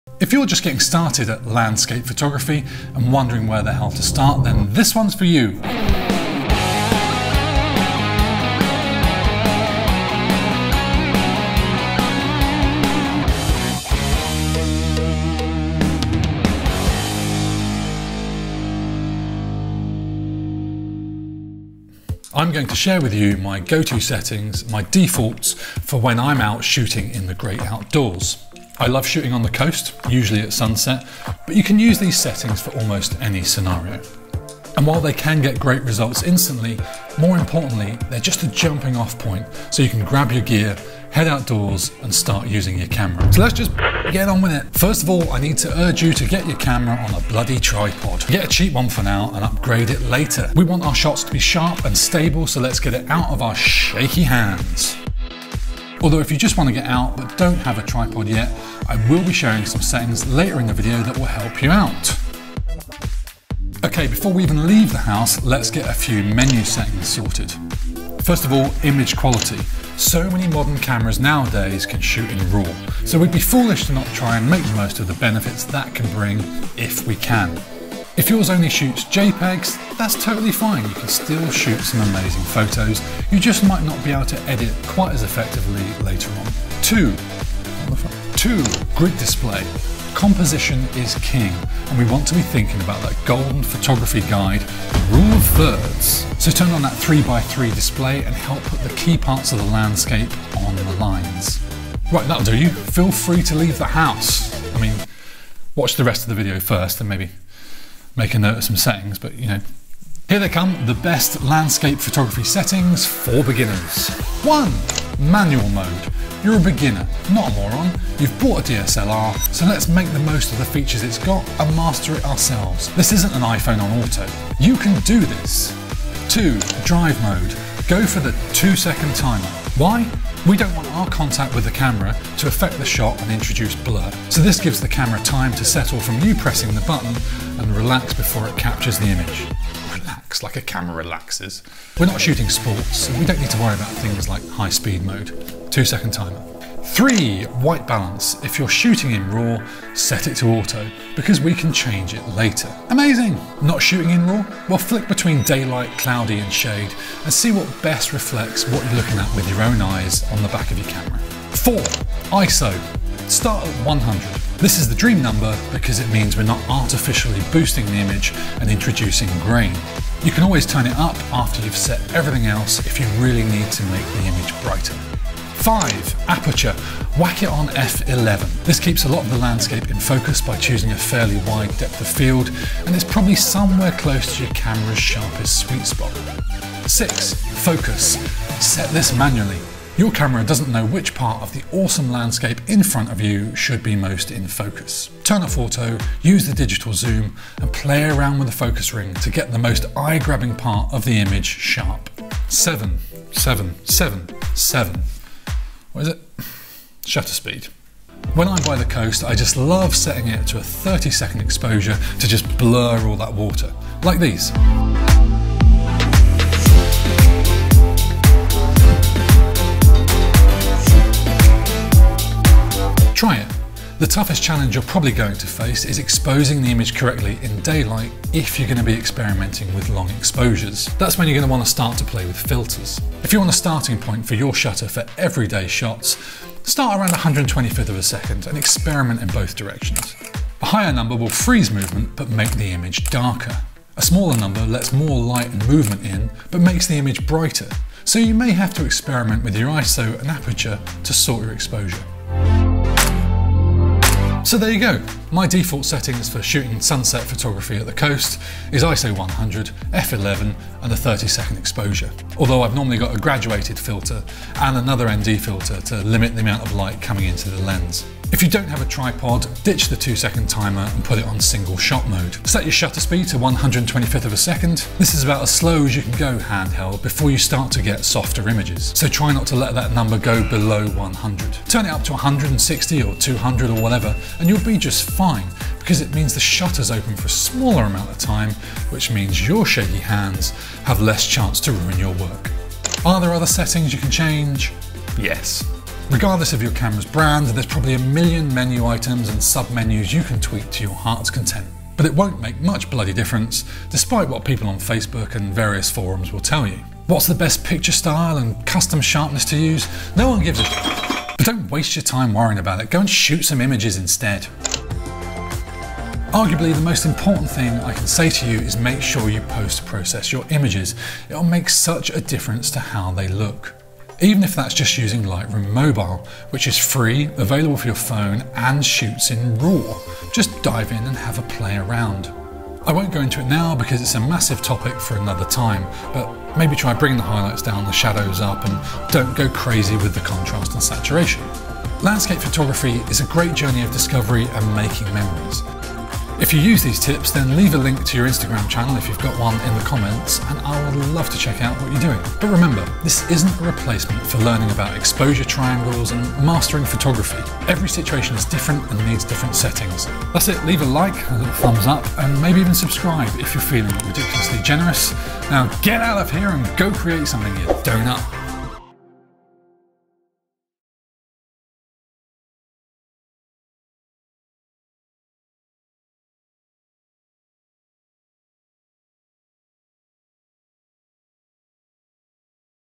The best landscape photography settings for BEGINNERS
Bought yourself a DSLR? Not sure where to start?
In this video I'll share 7 settings to get you started when you're shooting the great outdoors, along with a few extra tips and menu tweaks.
It doesn't matter whether you're a Canon, Nikon or Sony user (or anything else for that matter) - these are a starting point for you to jump off from.